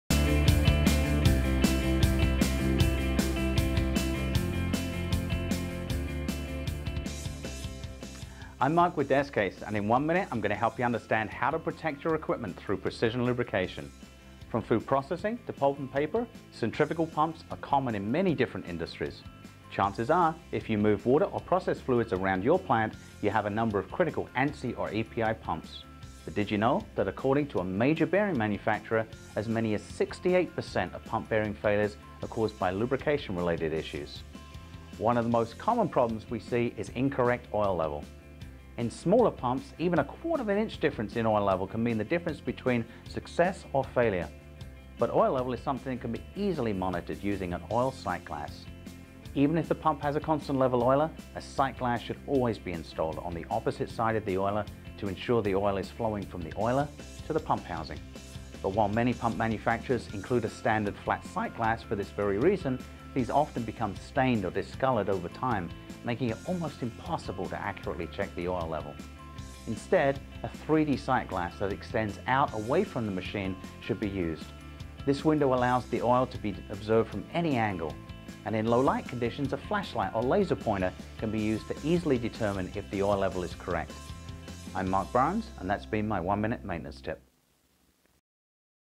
Des-Case Maintenance Tip - Avoid Pump Breakdowns Through Precision Lubrication
Learn how to avoid costly breakdowns from lubrication related issues and pump bearing failures.